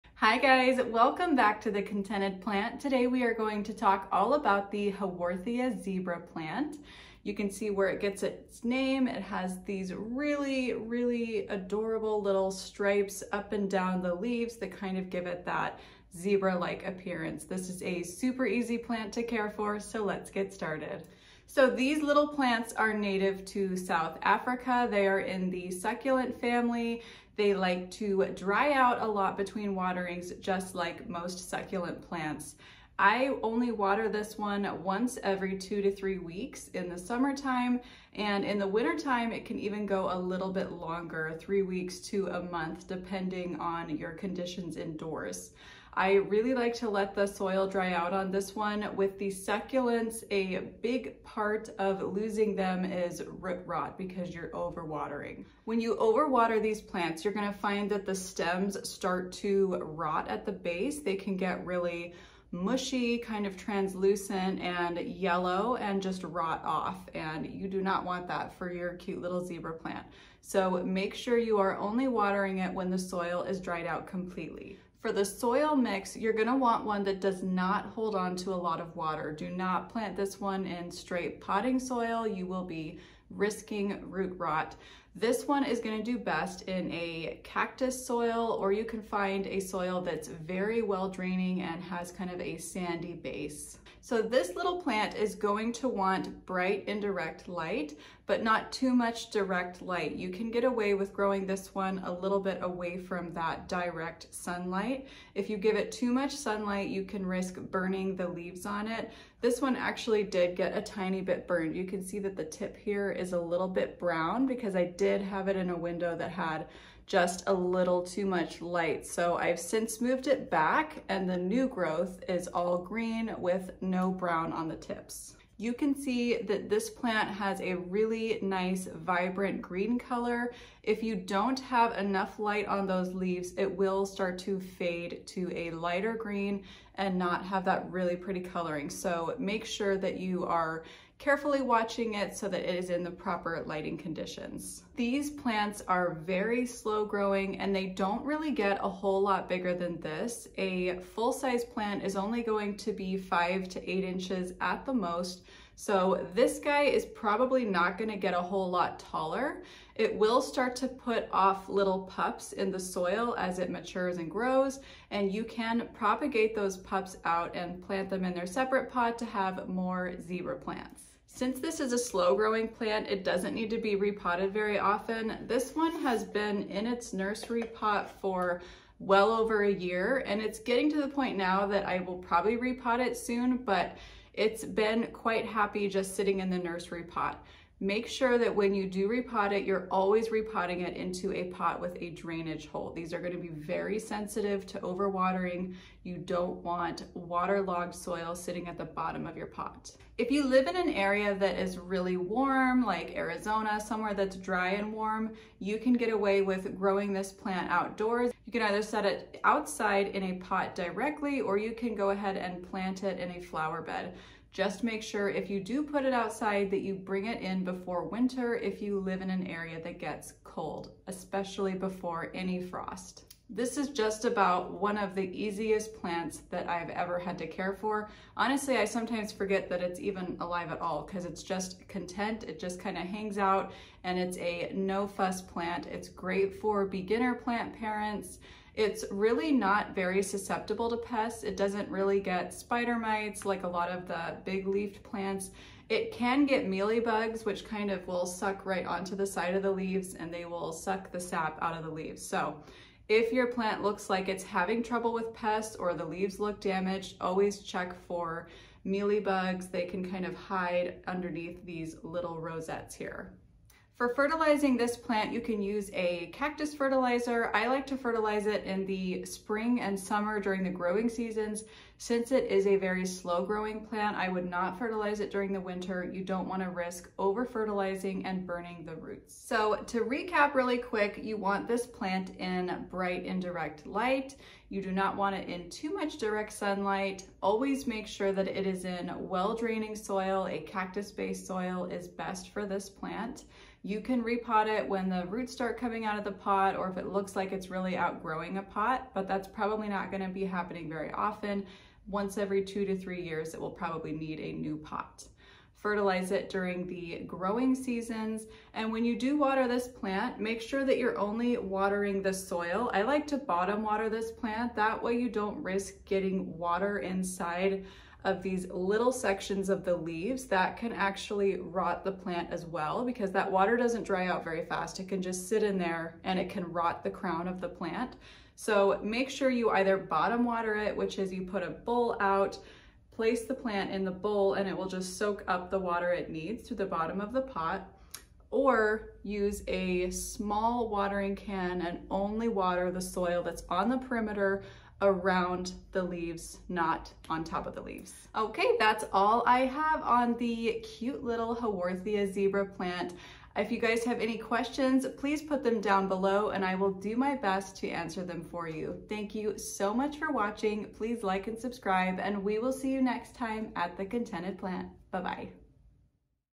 Haworthia Zebra Plant Care
Let's talk about the adorable Zebra plant! This fun little plant is easy to care for and great for beginners.